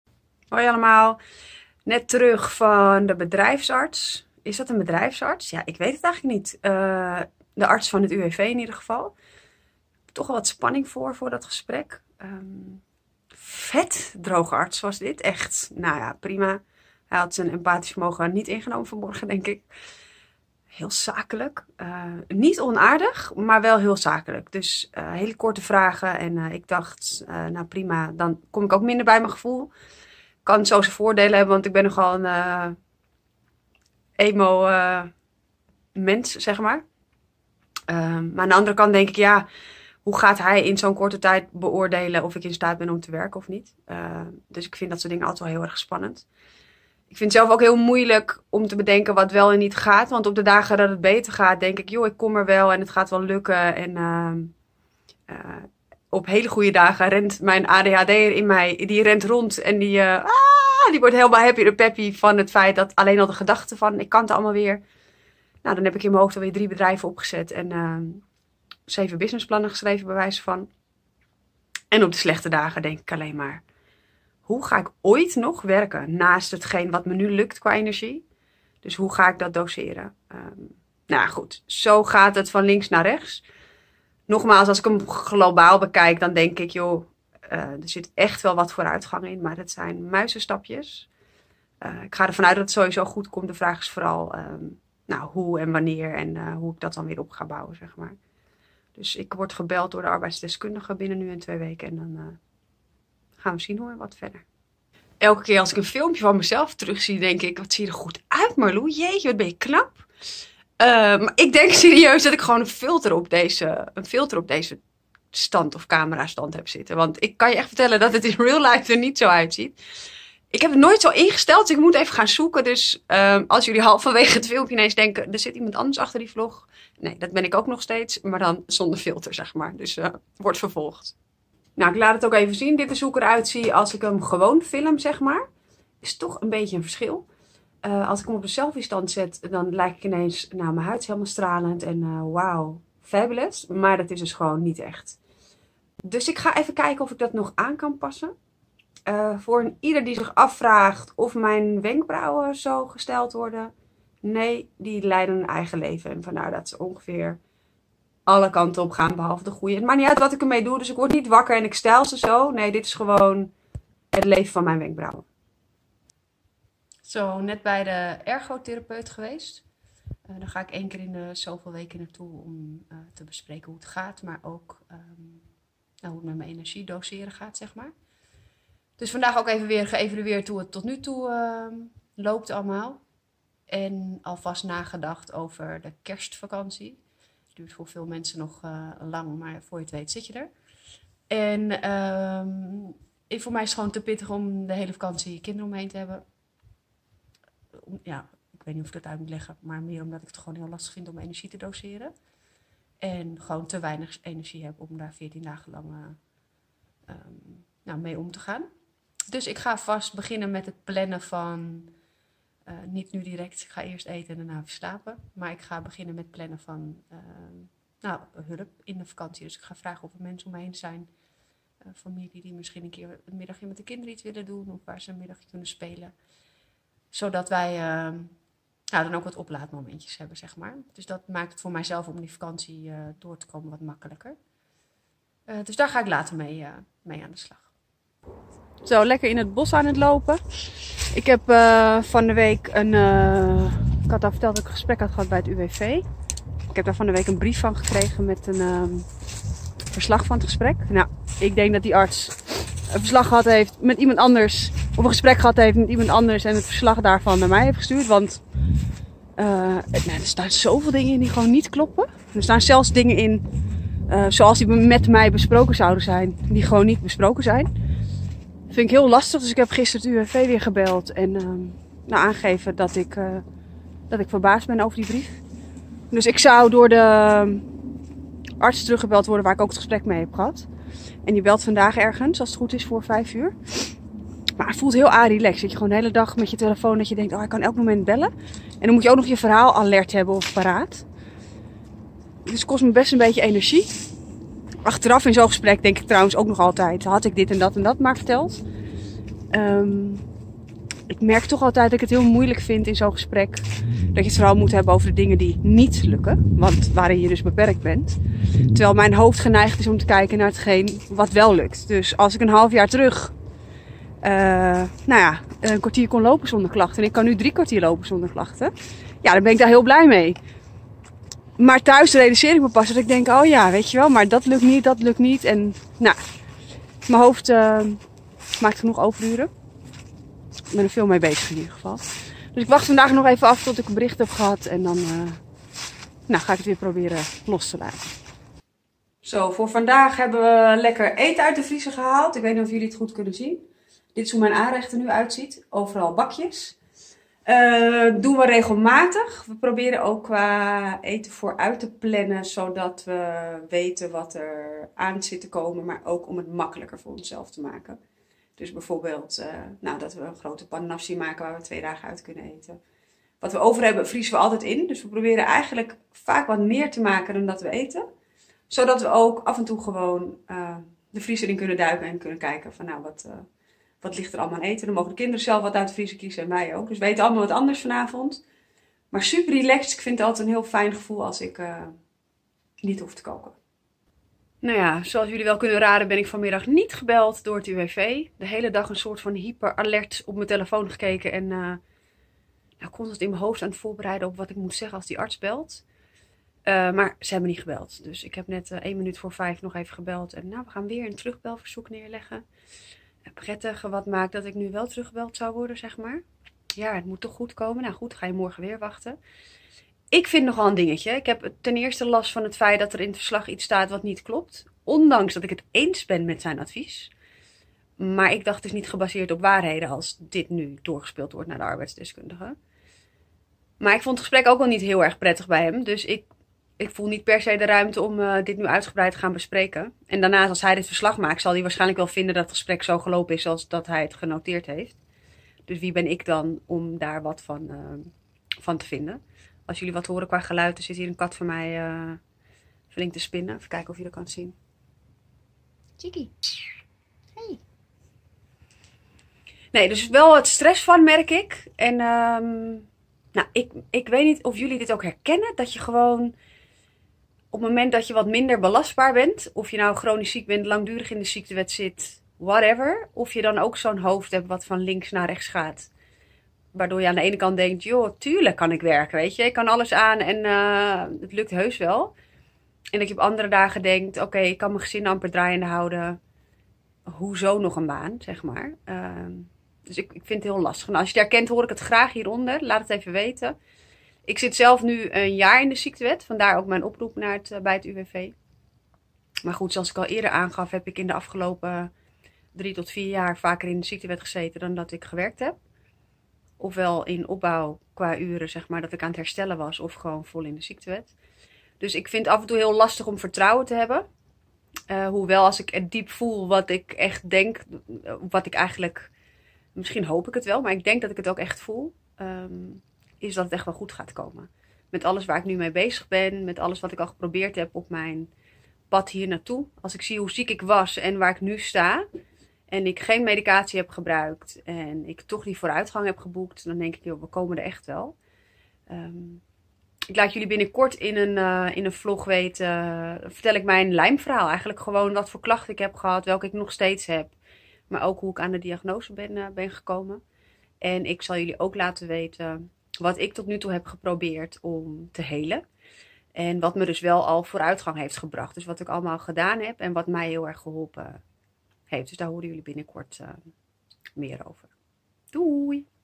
Weekvlog #5 UWV STRESS, GOED VOOR JE HERSTEL? | Marlou Aarding
🌀Openen voor meer informatie! 🌀

Op het moment van deze vlog zit ik een jaar in de ziektewet. En omdat ik vanuit de WW in de ziektewet ben gekomen, wordt je dan opgeroepen bij het UWV... Het loopt niet echt heel soepel ;)

Note to self: horizontaal vloggen Marlou, Horizontaal. Niet af en toe, maar structureel. Dank u.

🌀Social media🌀

🌀Benieuwd naar de overige vlogs? 🌀

Bedankt voor het kijken en tot volgende week !